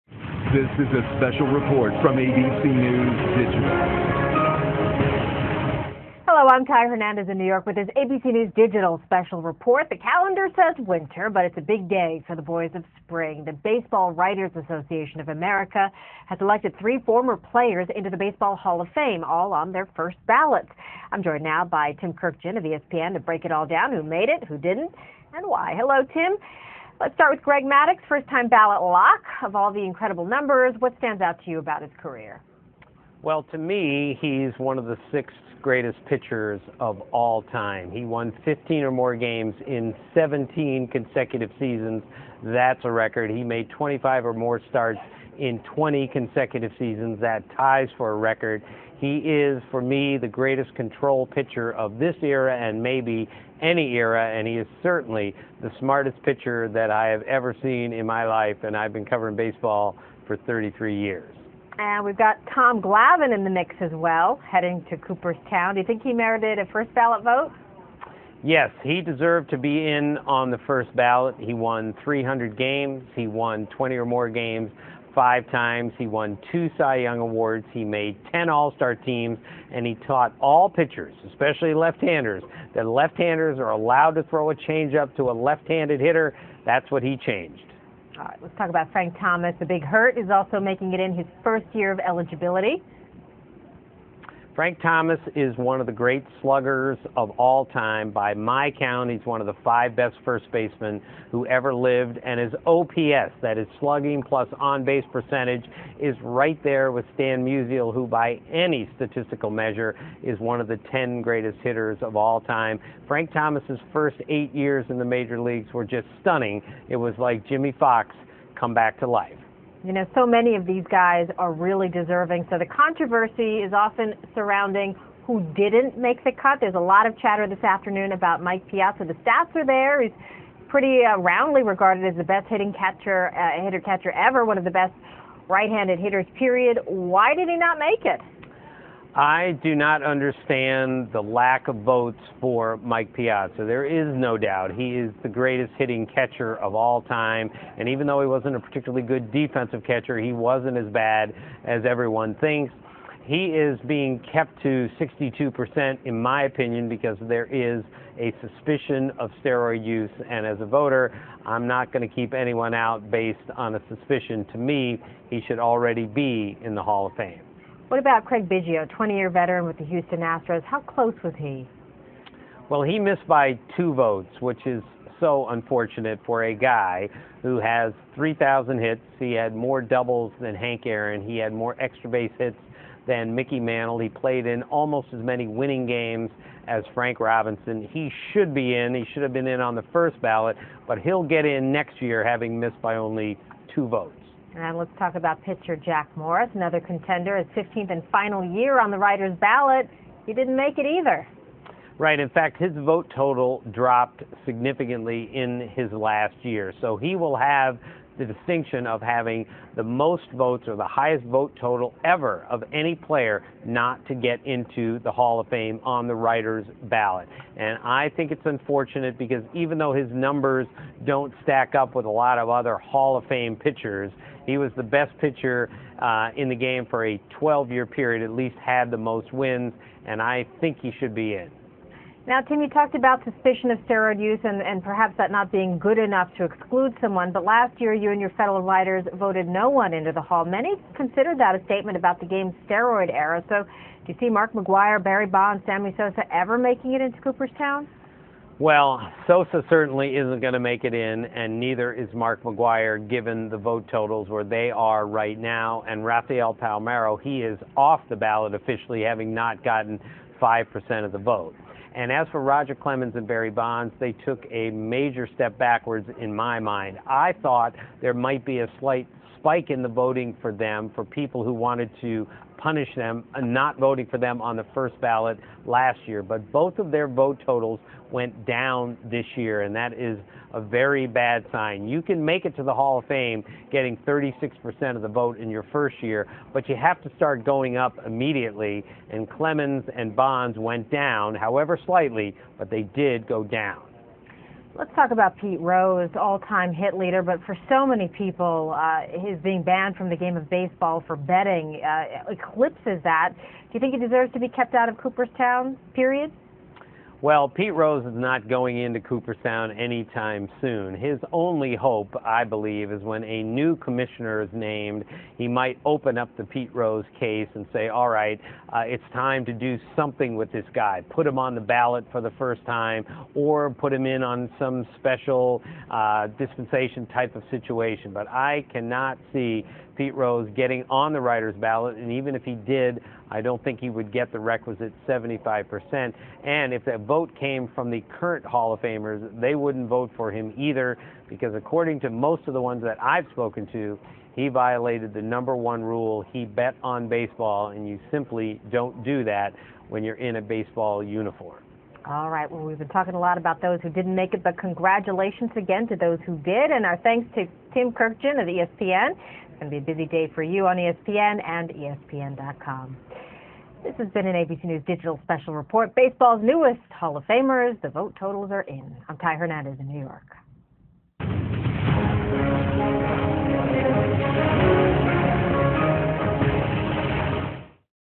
Former Atlanta Braves Aces Lead Way in Hall of Fame Voting
Greg Maddux, Tom Glavine join former White Sox slugger Frank Thomas as top vote getters in Hall of Fame voting.